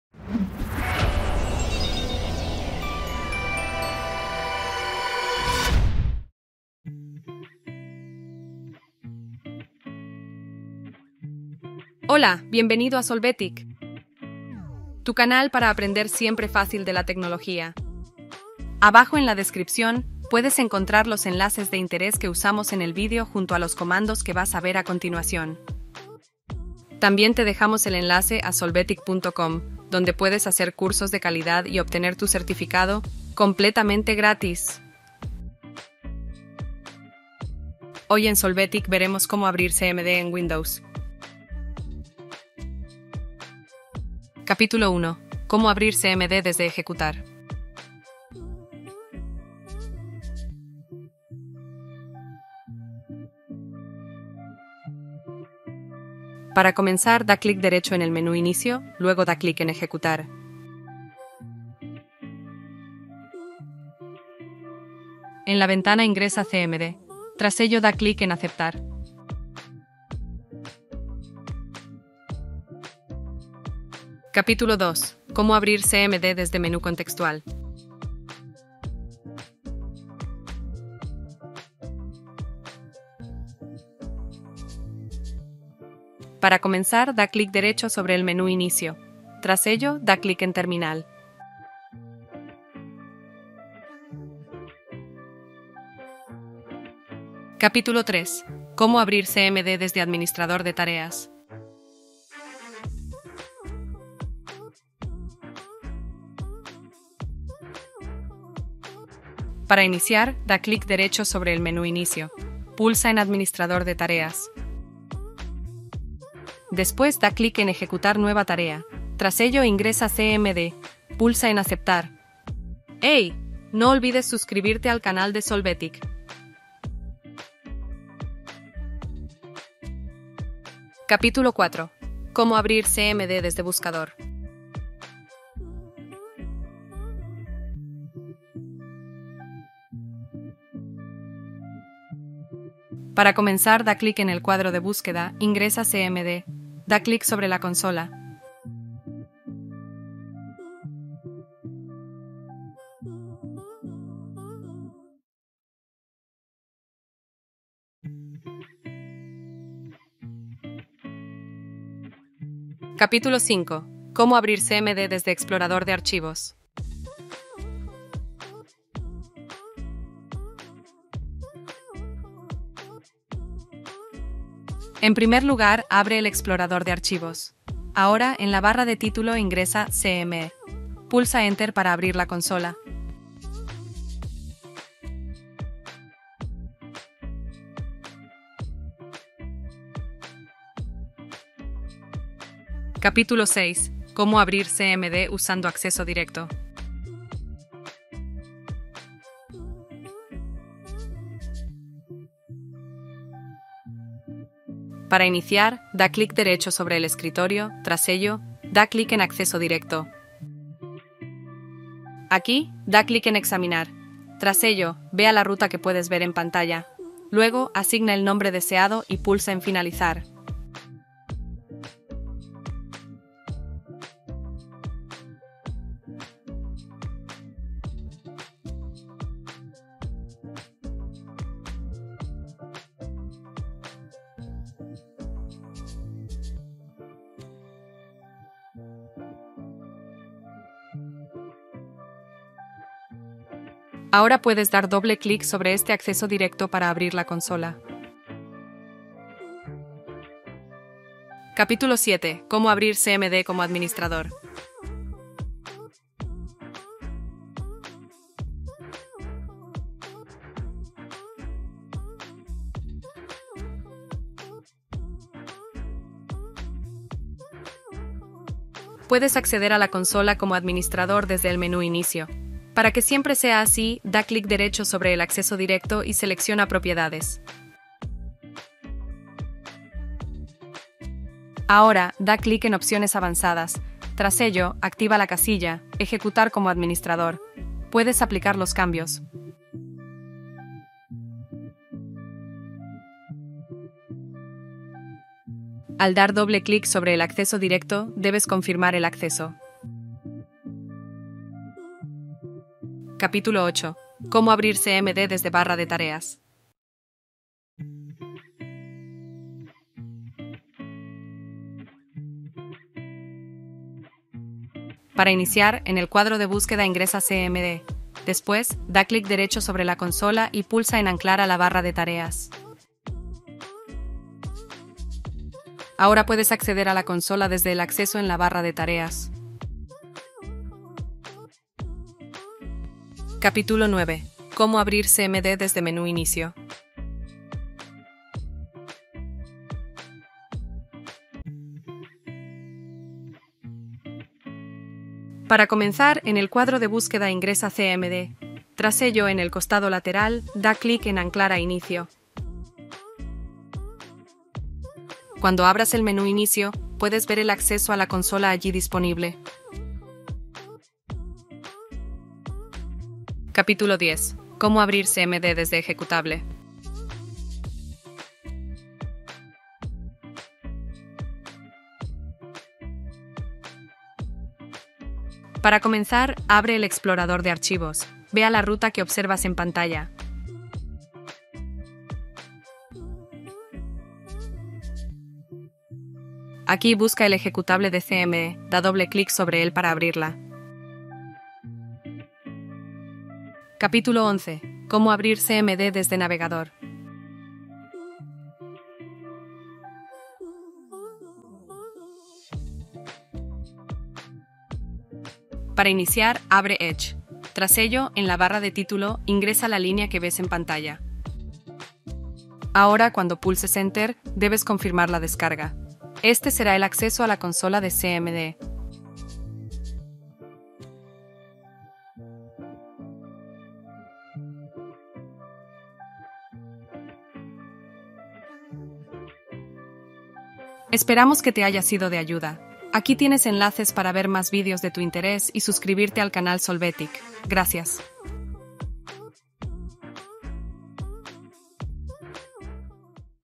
COMO PONER CMD
El la herramienta poner CMD en Windows nos ayuda a poder ejecutar multitud de tareas fácilmente. Es una interfaz de texto que permite ejecutar comandos y realizar tareas de administración del sistema. Dentro de saber abrir CMD Windows 11 podemos hacer una gestión de archivos y configuraciones, y el inicio de programas desde la línea de comandos. Abrir CMD como administrador es esencial para realizar tareas de administración y solucionar problemas en sistemas Windows. Entre las diferentes opciones podrás saber cómo abrir CMD desde una carpeta o abrir CMD con teclas.

 》CAPÍTULOS《

00:00 Introducción
00:45 Cómo abrir CMD desde Ejecutar
01:15 Cómo abrir CMD desde Menú contextual
01:39 Cómo abrir CMD desde Administrador de tareas
02:13 Cómo abrir CMD desde Buscador
02:43 Cómo abrir CMD desde Explorador de archivos
03:13 Cómo abrir CMD usando Acceso directo
04:12 Cómo abrir CMD como Administrador
05:03 Cómo abrir CMD desde Barra de tareas
05:33 Cómo abrir CMD desde Menú Inicio
06:04 Cómo abrir CMD desde ejecutable
06:35 Cómo abrir CMD desde navegador